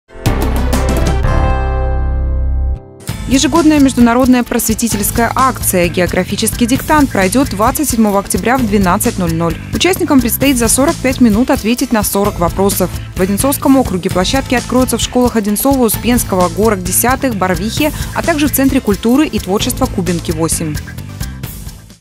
Просветительская акция
27 октября в 12:00 жители Одинцовского округа смогут написать географический диктант